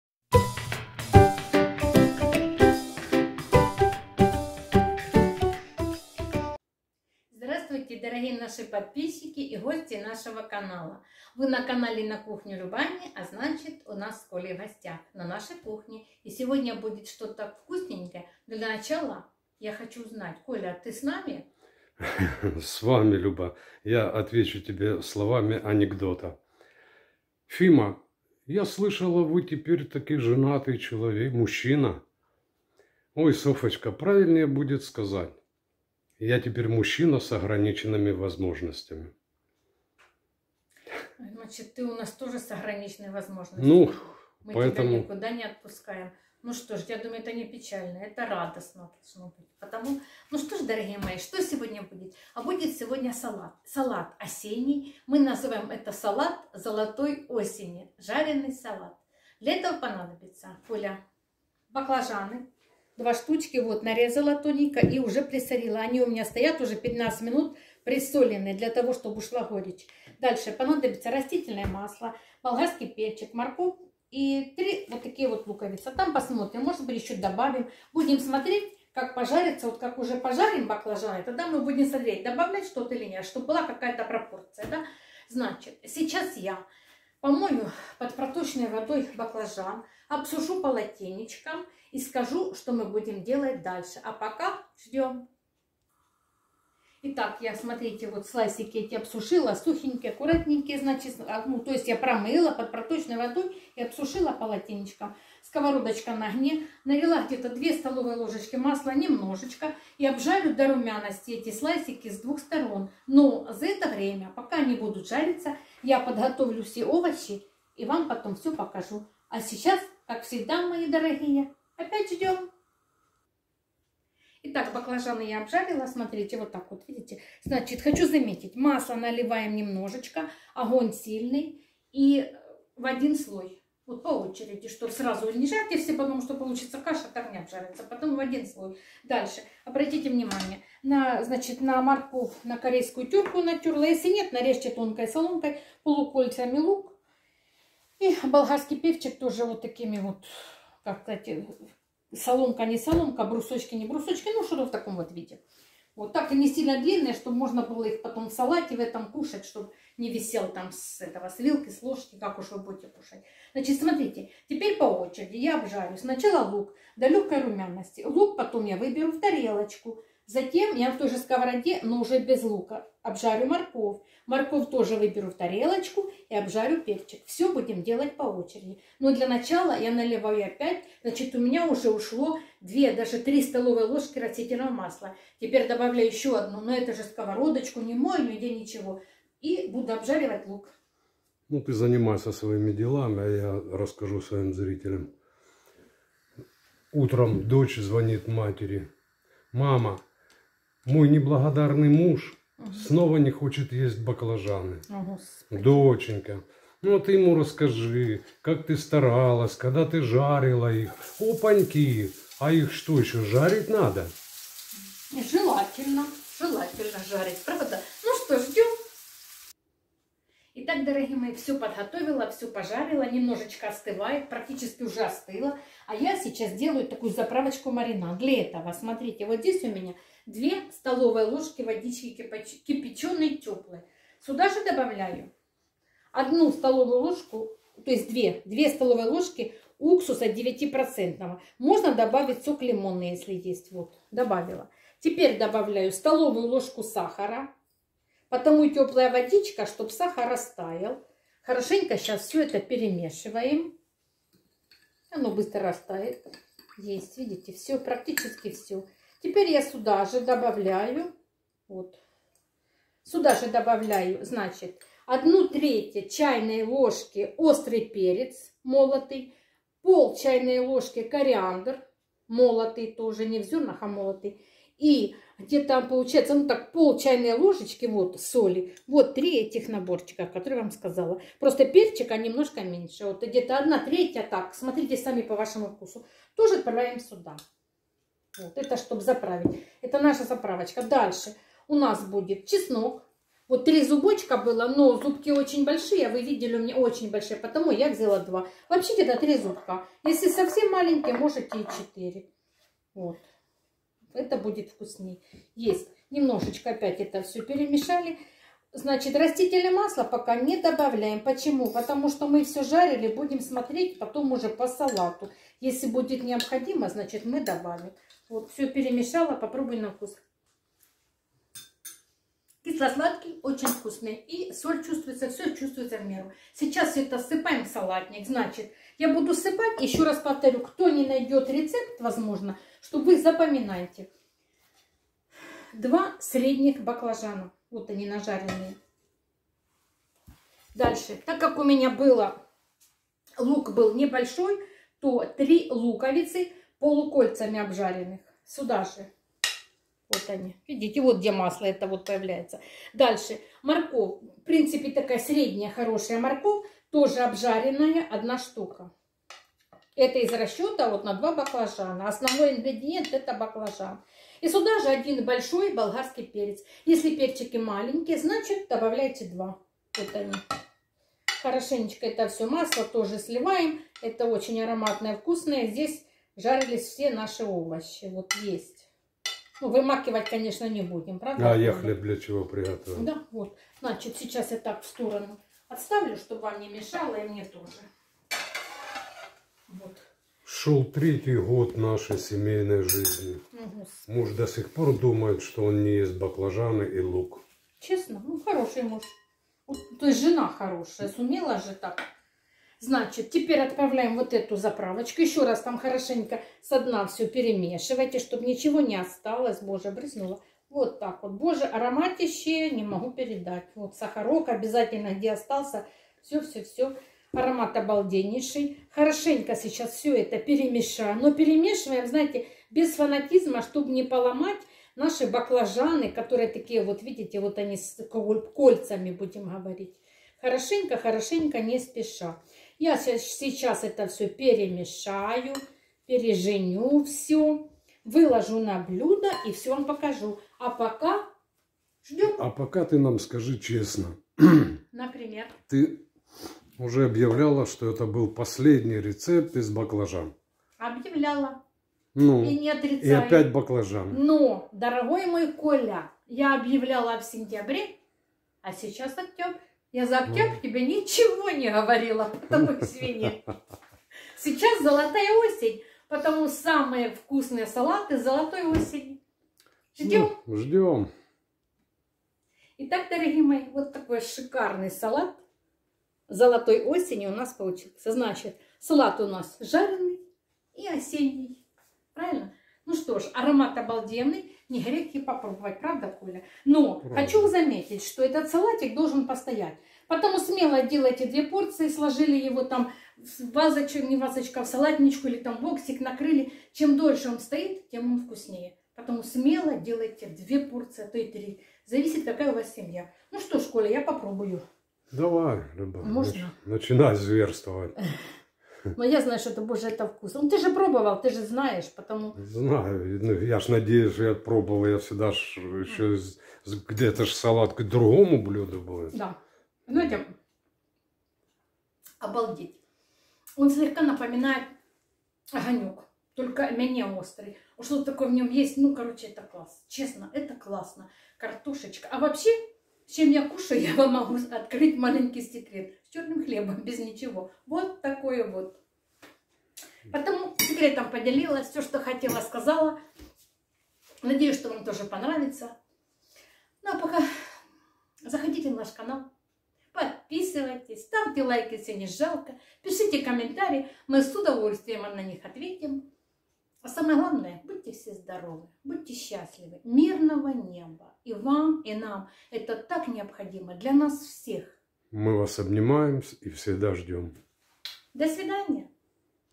Салат золотой осени! Жареный салат!
Золотая осень! Прекрасный жареный салат с аналогичным названием приятно удивит вас и ваших гостей, своим неповторимым вкусом!
  Баклажаны, морковь, болгарский перец, лук и специи - вот что нужно в эту осеннюю золотую пору.

              ИНГРЕДИЕНТЫ:
баклажан     2 шт.
растит. масло для жарки всех овощей      5-7 ст. л.
  ЗАПРАВКА:
 уксус 9%     2 ст. л.( можно заменить лимонным соком).
соль       0.5 ч. л. ( по вкусу).
перец чёрный молотый      0.5 ч. л. ( по вкусу).
кориандр ( молотый)       0.5 ч. л. ( по вкусу).
чеснок      2-3 зубка.
петрушка   по вашему вкусу.
    ПРИЯТНОГО АППЕТИТА!